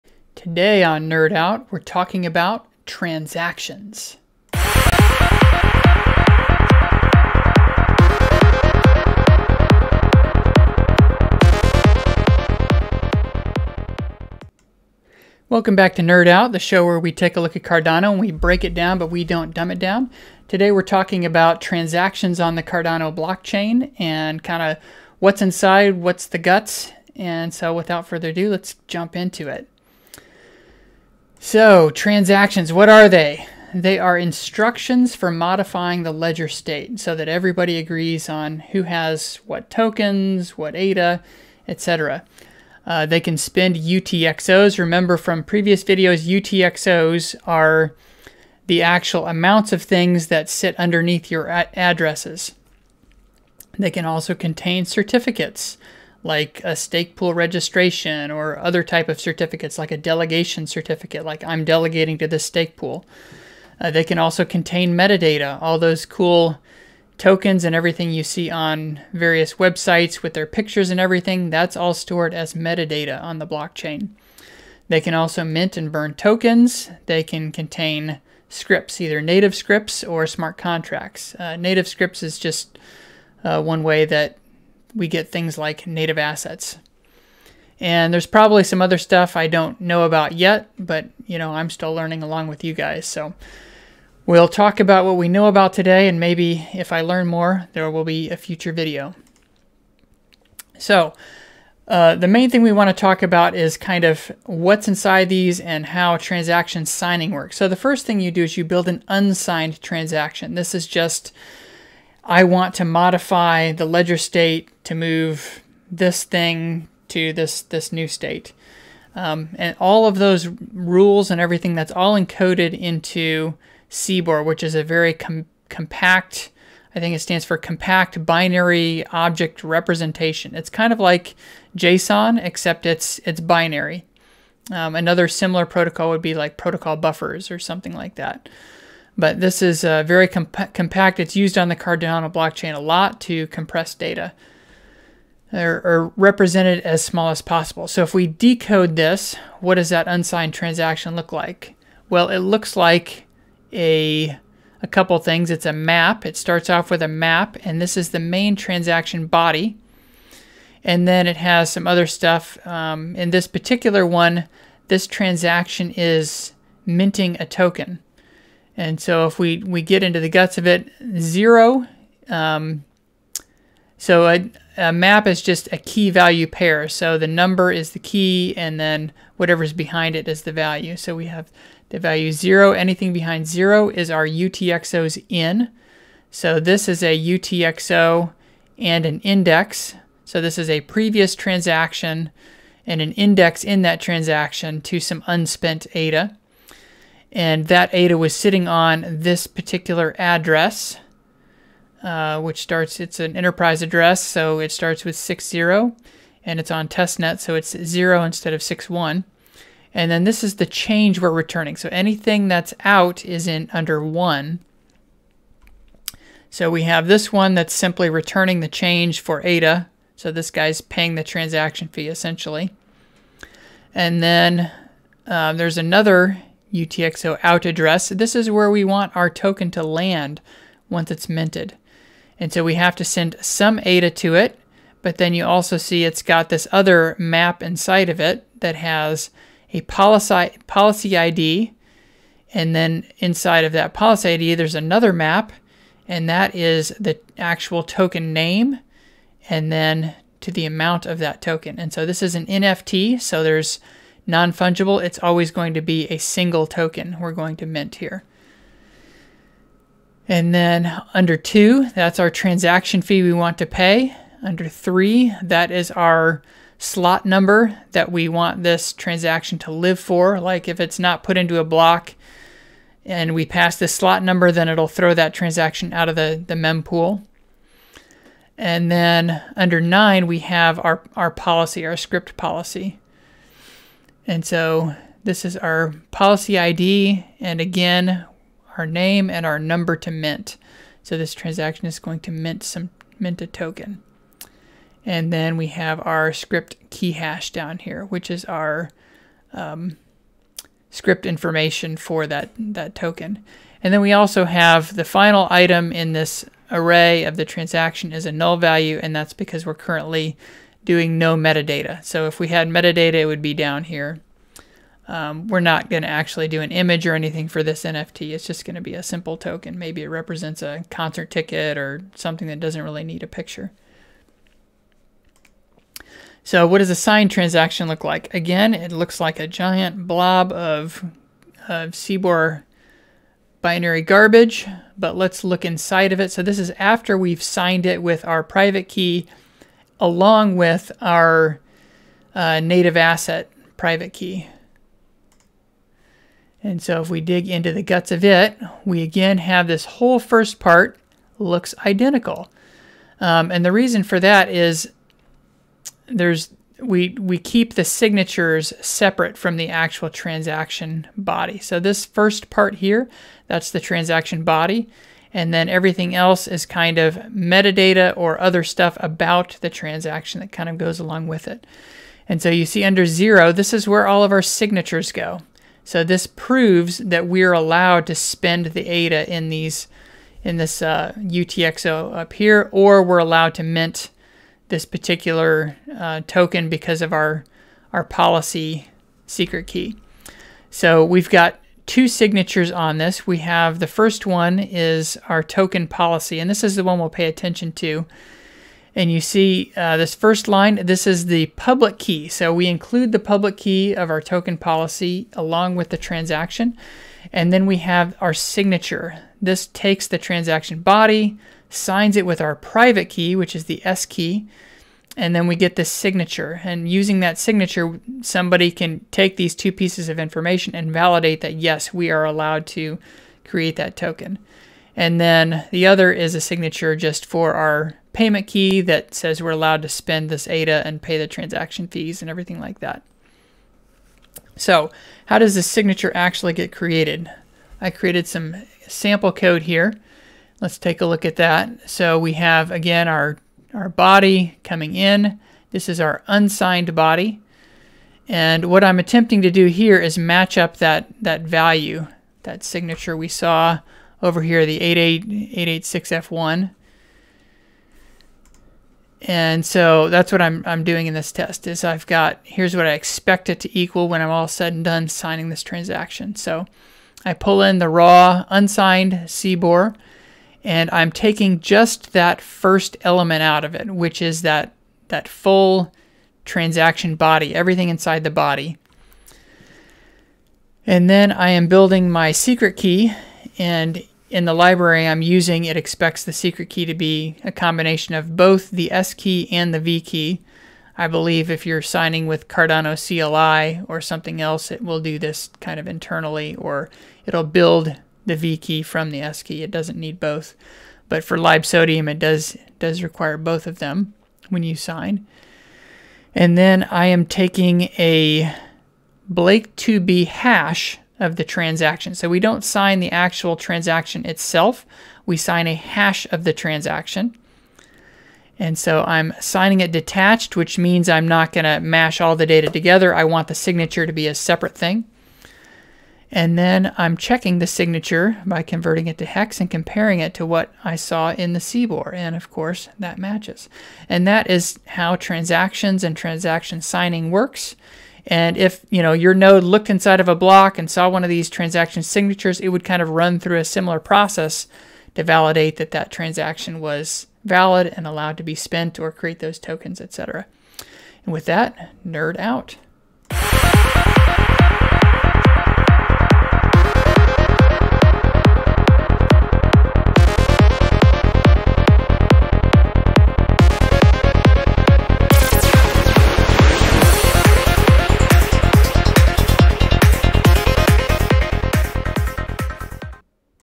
Cardano: NerdOut - Transactions
Learn what is inside the guts of a Cardano Transaction.